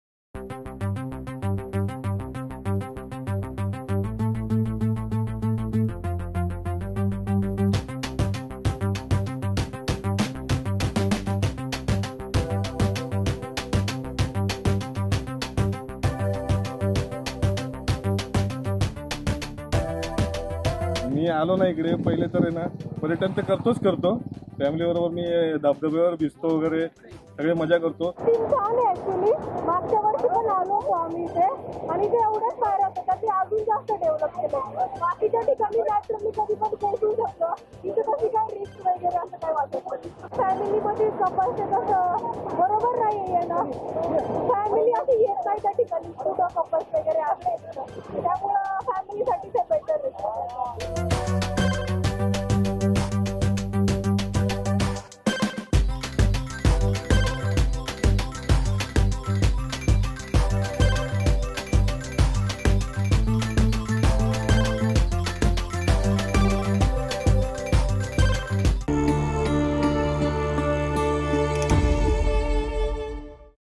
Maval waterfall attracts tourists
Lalwadi waterfall and mau waterfall in Maval are attracting tourists. The tourists coming from cities like Pune and Mumbai are enjoying these waterfalls.